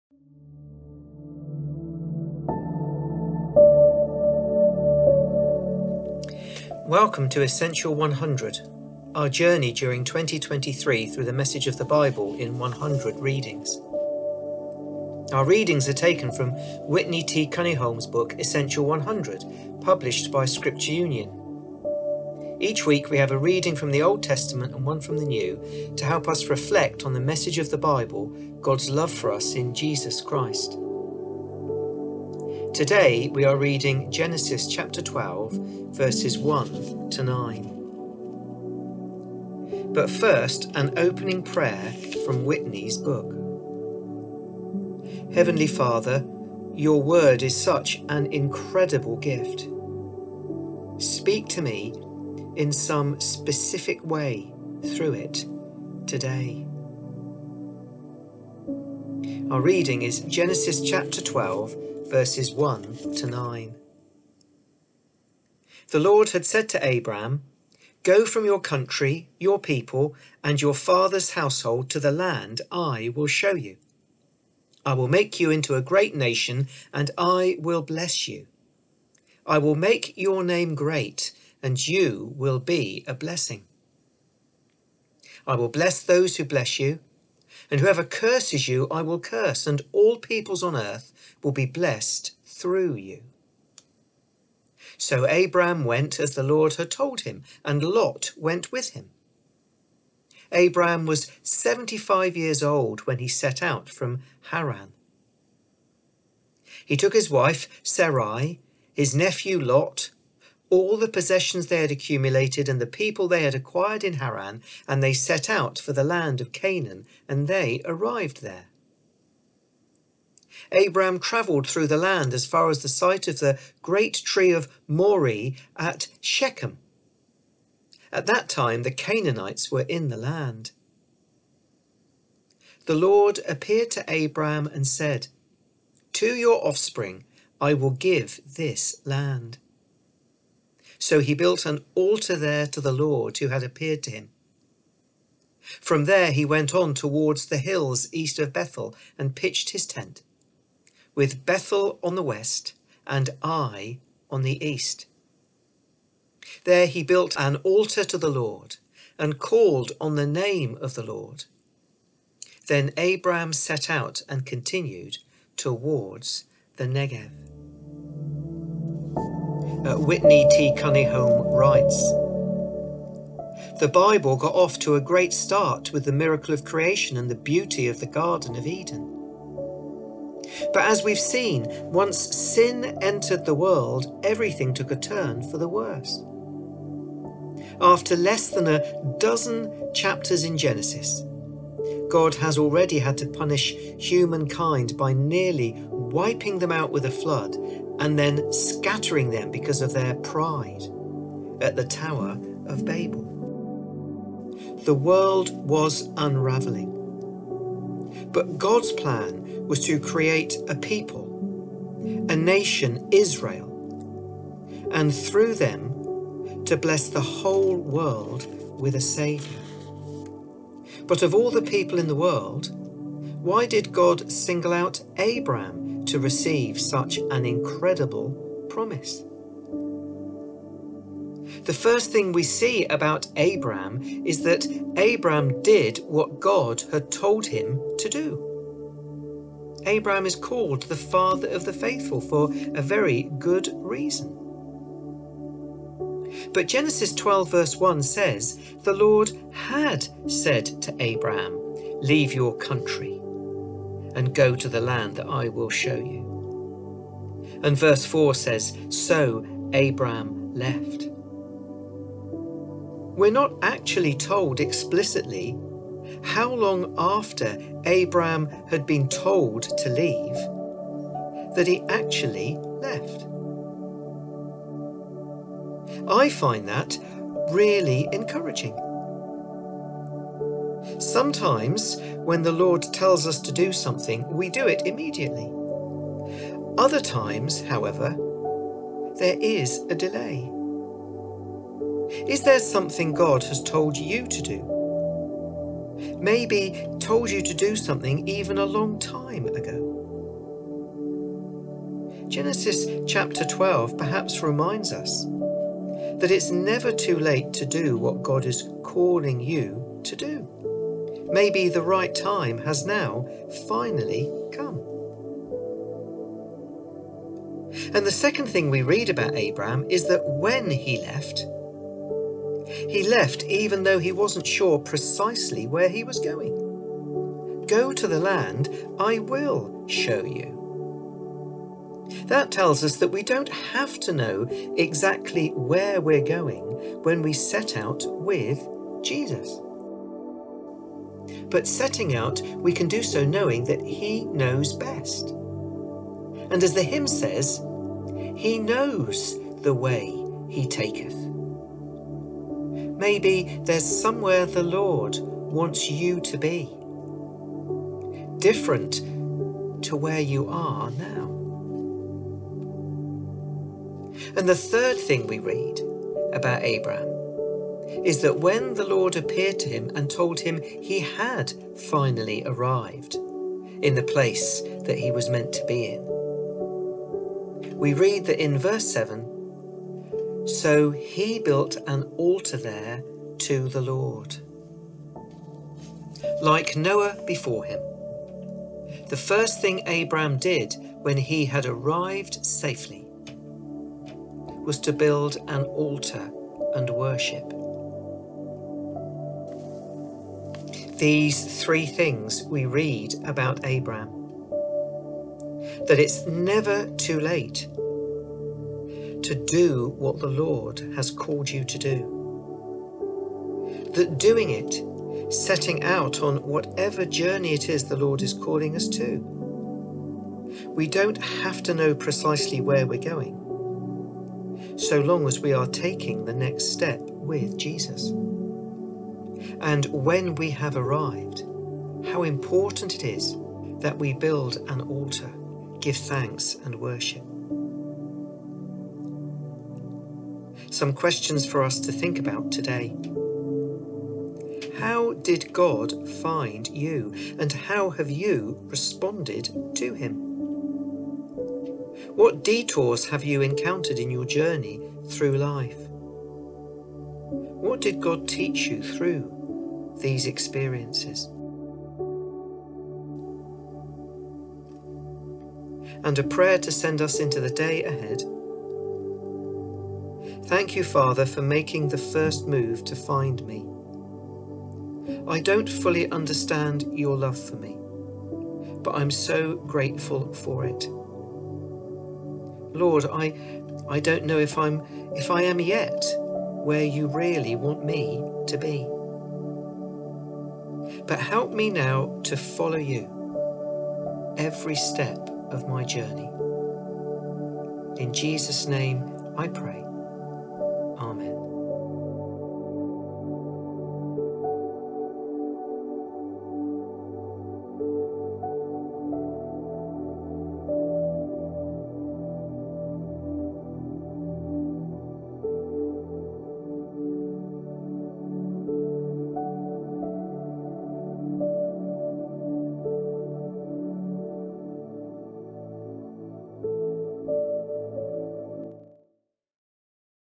Essential 100 Bible Reading 7th February 2023, Westborough & Wonersh URCs & Emmanuel Church Normandy
Our Video and Audio Essential 100 Bible Readings continue with readings for the week beginning Monday 6th February 2023. Today Graham Hoslett reads Genesis 12:1-9.